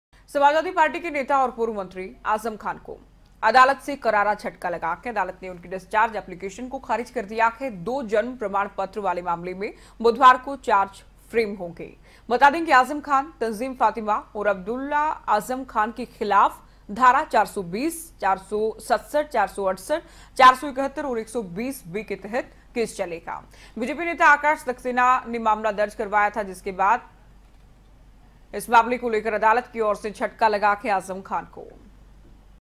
आजम खान को फिर लगा करारा झटका, कोर्ट ने चार्ज Application को ठुकराया
आजम खान को फिर लगा करारा झटका,  कोर्ट ने चार्ज Applications को ठुकराया